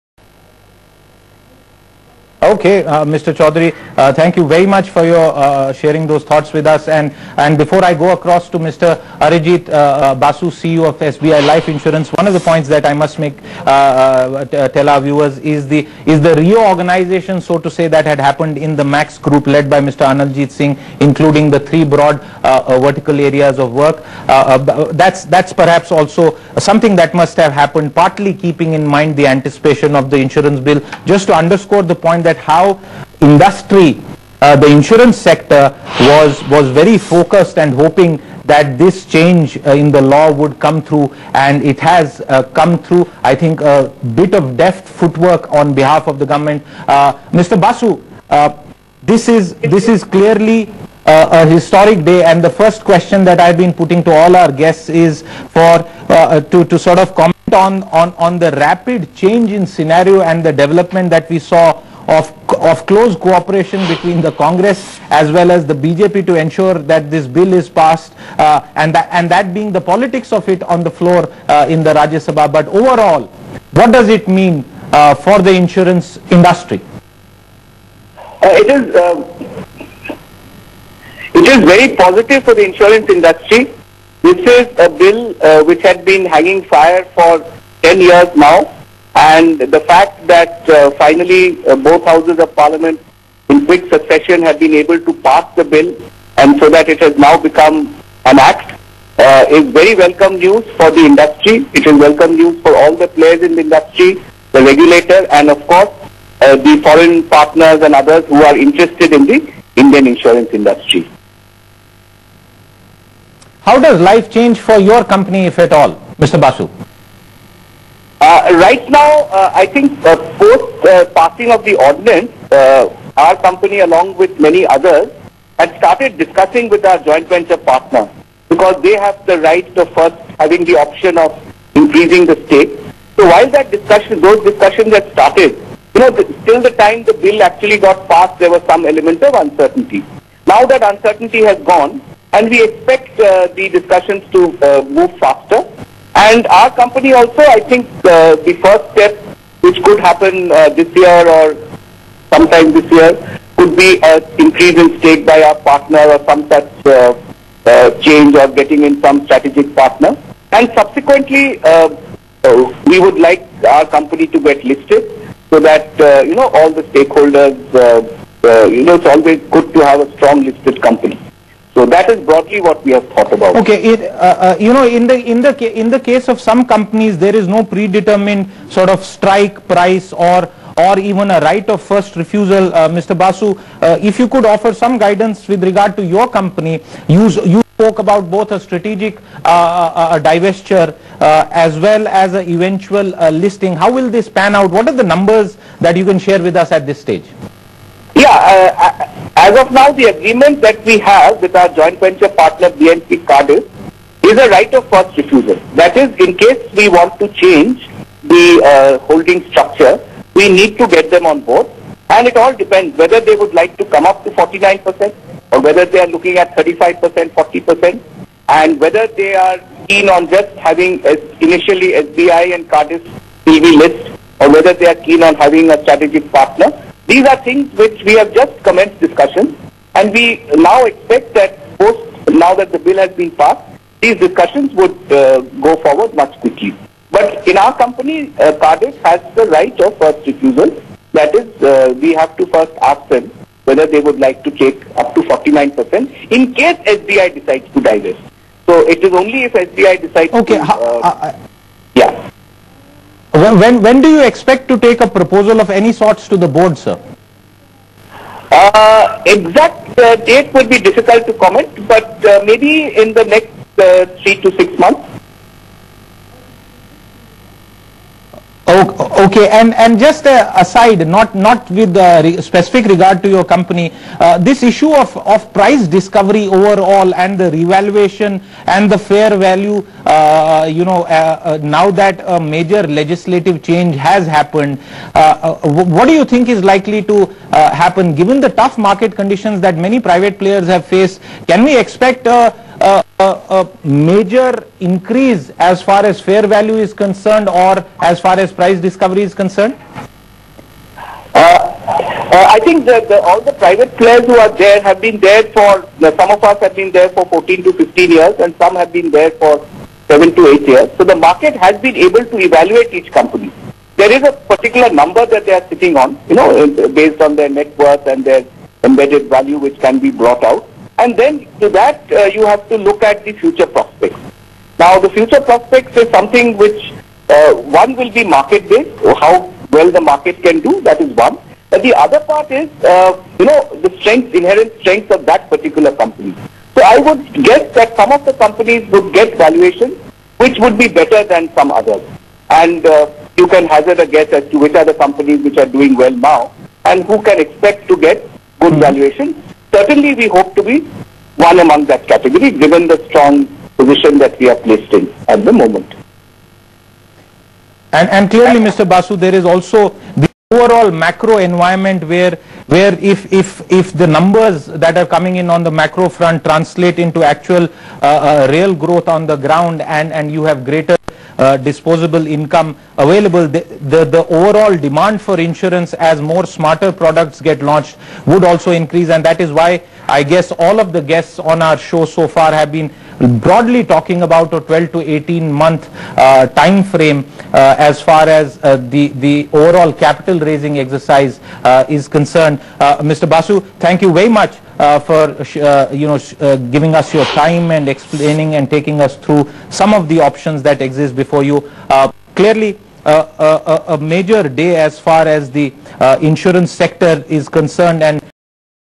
Bloomberg TV interview with Mr. Arijit Basu, MD & CEO, SBI Life.
Bloomberg TV interview with Mr. Arijit Basu, MD & CEO, SBI Life., expressing optimism for the sector post parliament passing the insurance bill